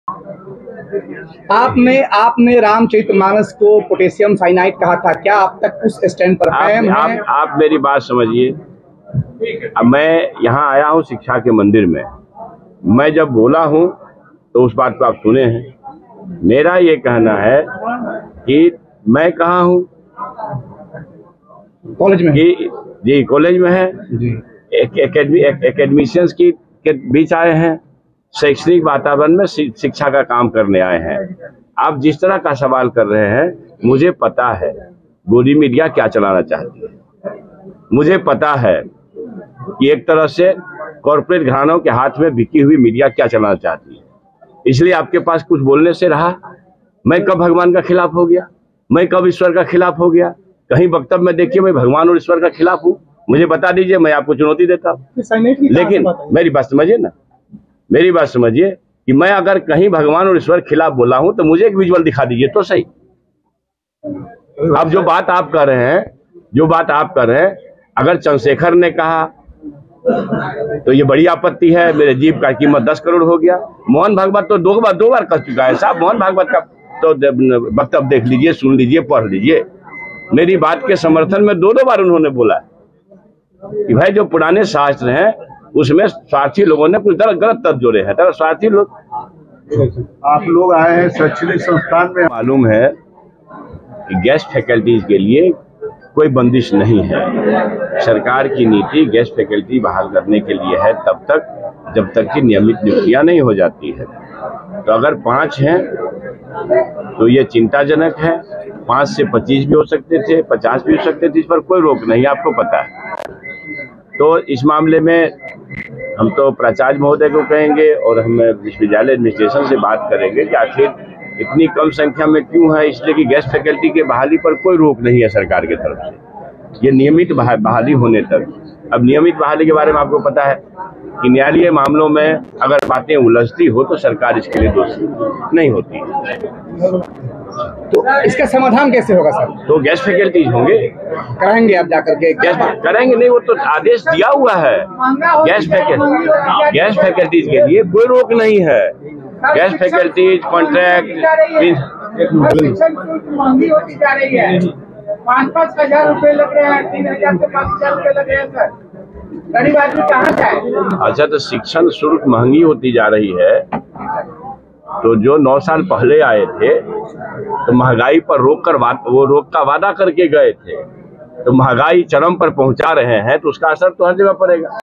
Bihar News : 'मैंने भगवान के खिलाफ कब बोला, Education Minister ने मारी पलटी | Breaking News
रामचरितमानस पर विवादित बयान को लेकर बिहार के शिक्षा मंत्री चंद्रशेखर लगातार सुर्खियों में बने हुए हैं. रामचरितमानस में पोटैशियम साइनाइड से लेकर भगवान राम के सपने में आने के बयान को लेकर चंद्रशेखर पर हमला हो रहा है. हालांकि अब उन्होंने अपने बयान से पलटी मार दी है .